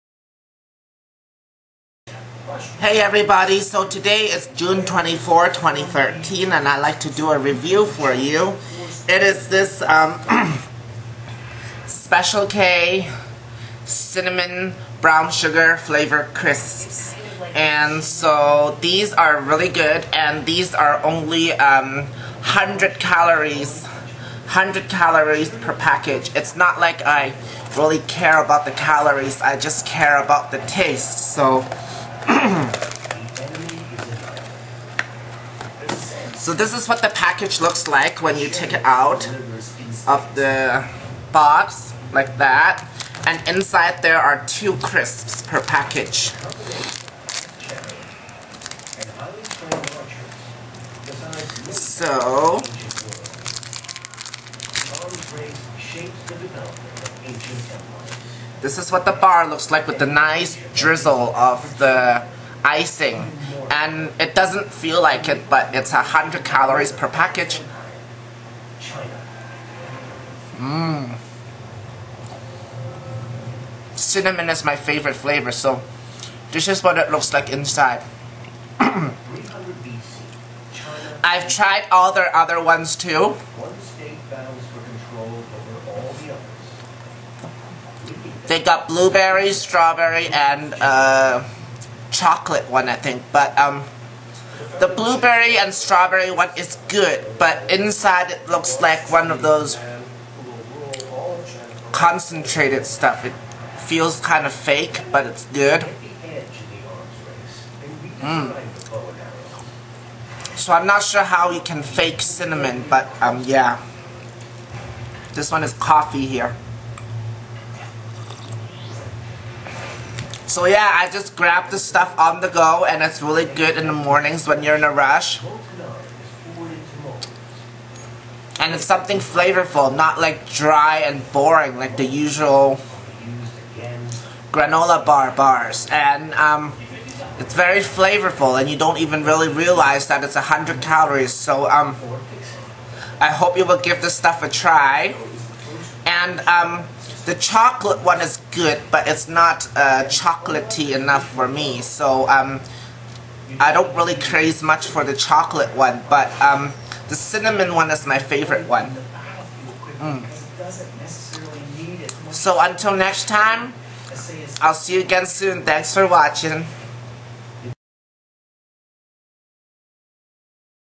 Review of - Special K Crisps Bars Cinnamon Brown Sugar Flavor
This stuff is so good!!!  I can eat a tonne of this stuff without thinking about the Calories.  100 Calories per 2 pieces.  Hope you guys enjoy!!!